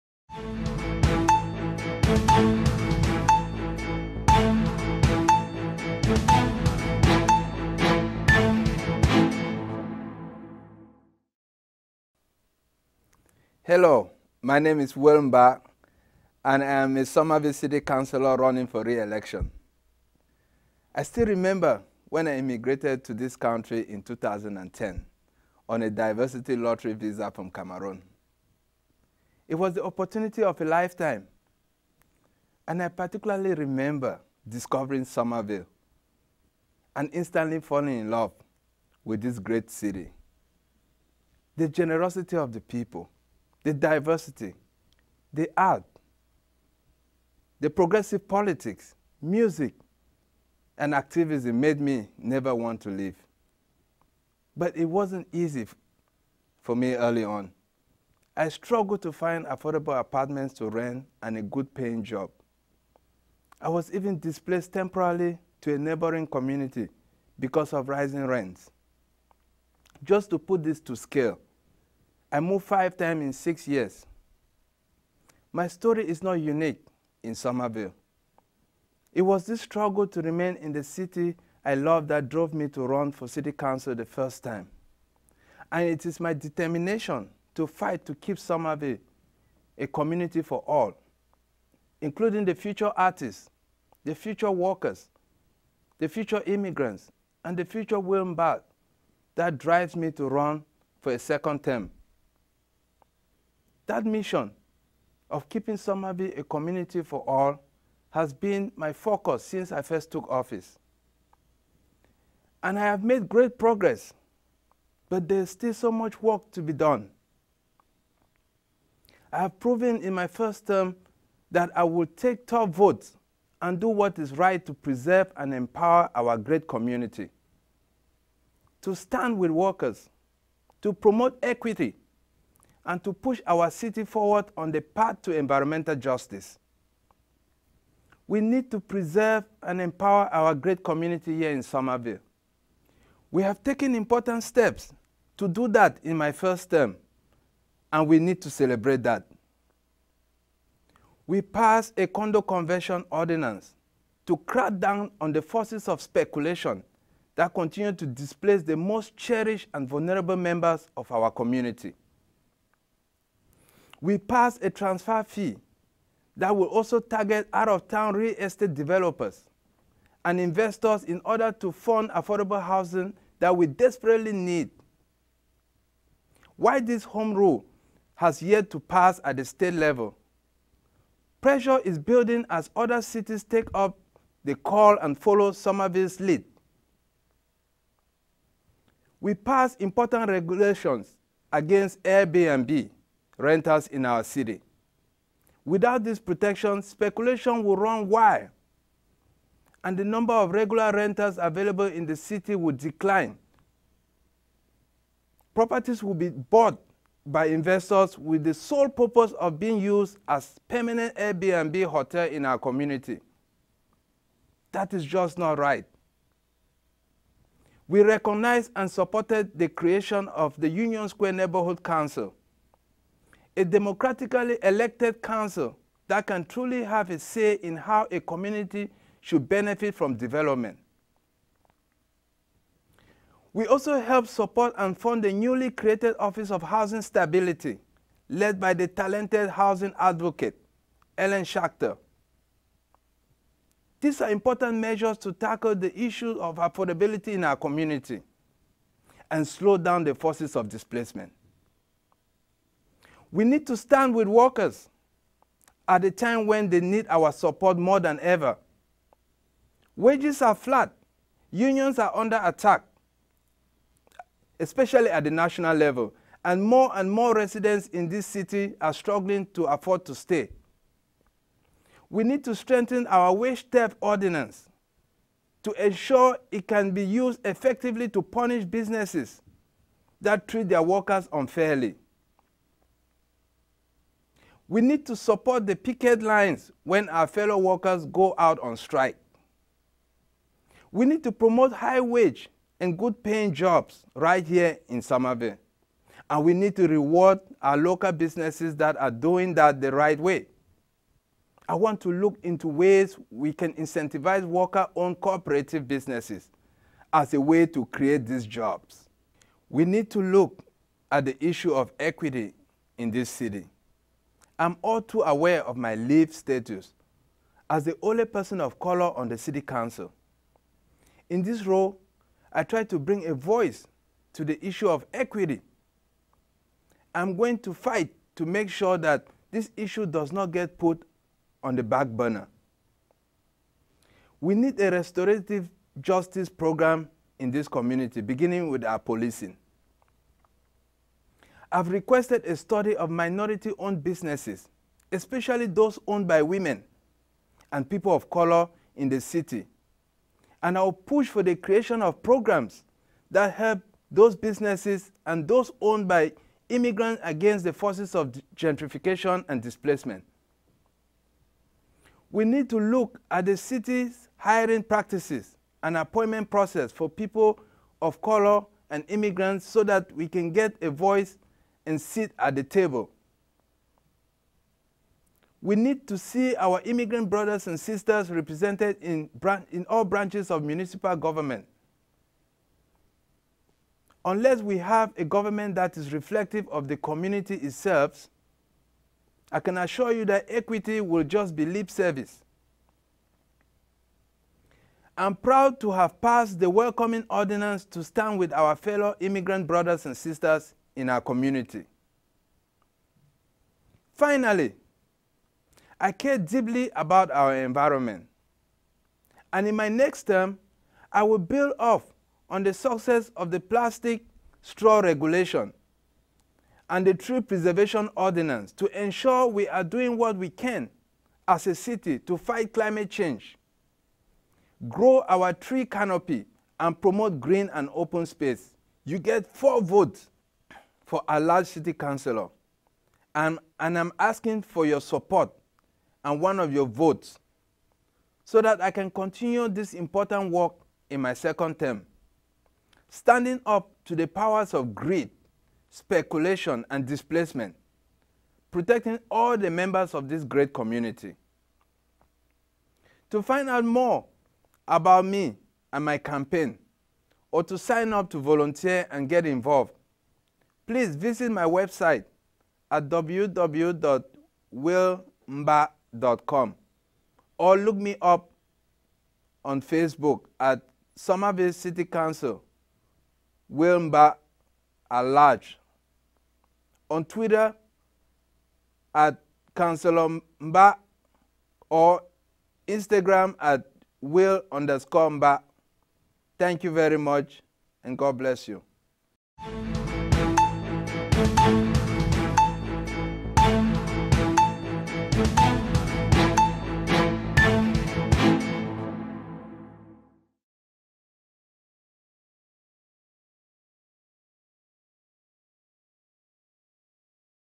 2019 Candidate Profile Video: Will Mbah
Somerville Media Center presents the 2019 Somerville Candidate Profile Series. SMC brings you the opportunity to get to know more about your local candidates running for political office.